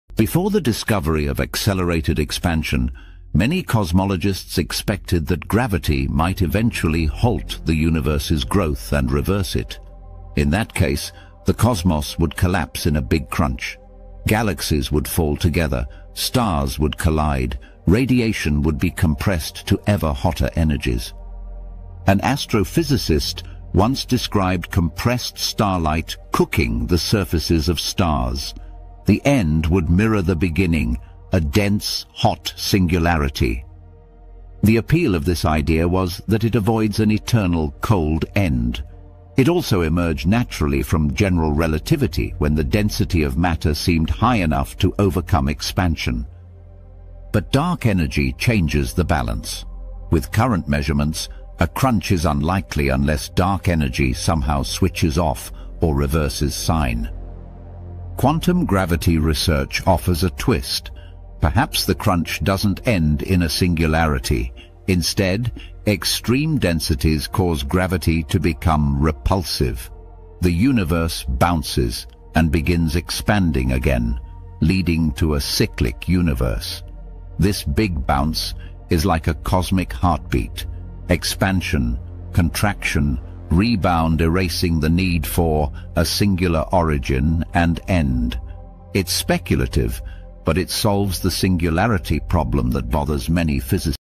Cosmic Heartbeat: The Universe's Endless Rebirth?
What if the cosmos is hurtling toward a cataclysmic end? This video explores the possibility of a "big crunch theory", a scenario where the "universe collapse" reverses into a hot, dense state. Discover the science behind this "cosmic crunch", and how it relates to "astrophysics", "space science", and "cosmology".